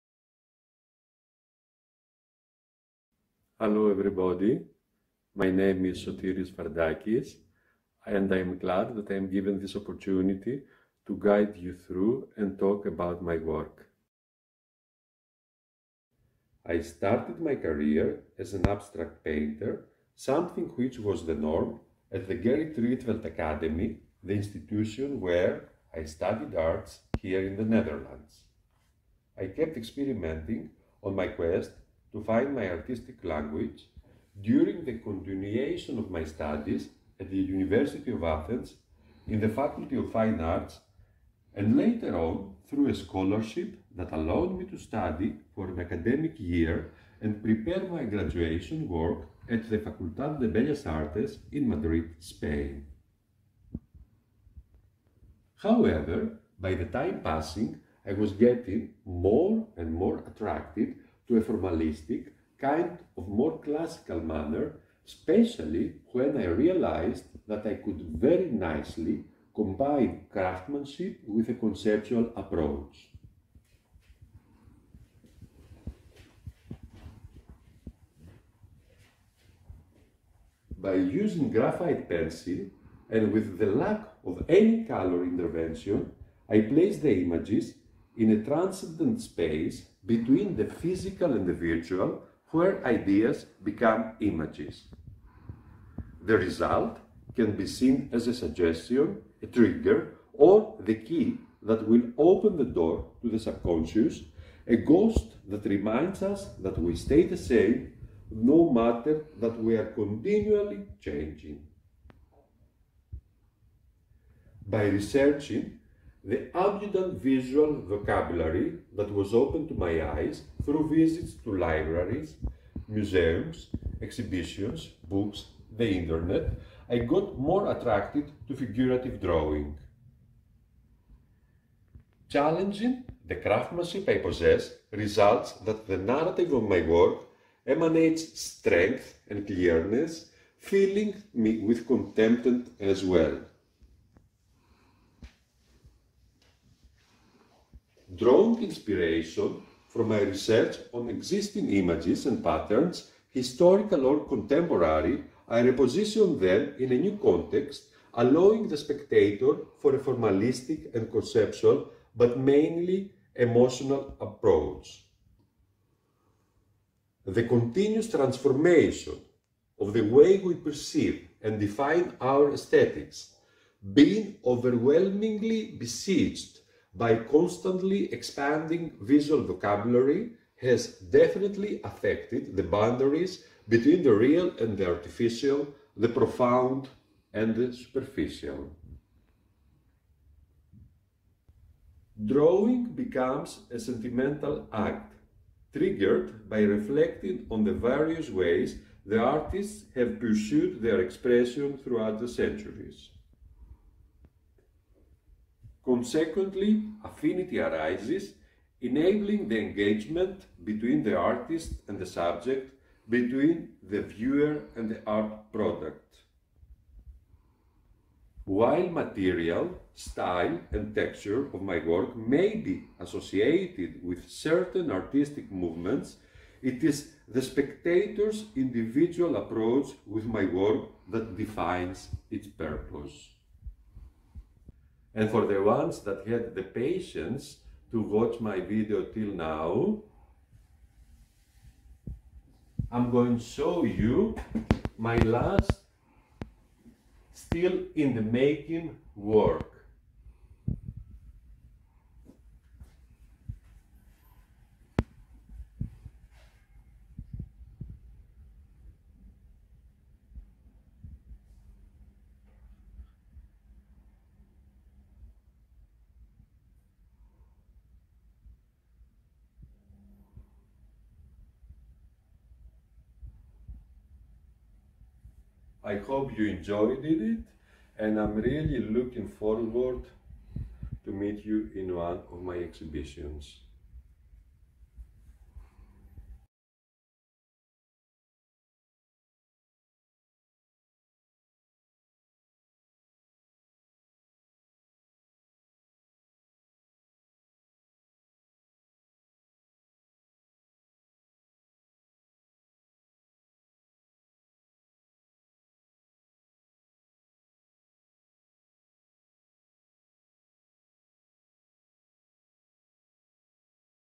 Artists in their studios, Sotiris Vardakis
Studies:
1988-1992    Gerrit Rietveld Academy/Art department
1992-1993    University of Athens Greece/ Faculty of Fine Arts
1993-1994    Scholarship in order to attend an academic year at the Complutense University of Madrid/ Faculty of Fine
                     Arts
1994             Master in the Arts, University of Athens Greece, Faculty of Fine Arts.
Exhibitions:
1992            Green Door gallery, Athens Greece
1993            Gallery/Cafe Amsterdam, Athens Greece
1994            Gallery Plaza Marina , Madrid Spain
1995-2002   Founding member of the Artists collective  "Cheap Art". Participating twice a year at the group exhibitions the
                    collective had been organizing in Athens Greece
2007            Kunstlerhaus, Vienna Austria
2008            Bloems en Co Gallery/Flower Shop Amsterdam Netherlands
2015            Exo Meria  Gallery/Cafe  Tinos Island Greece
2016            Philadelfeos Art Shop/Gallery Arnhem Netherlands

Works of Sotiris Vardakis have been acquired by various private collections.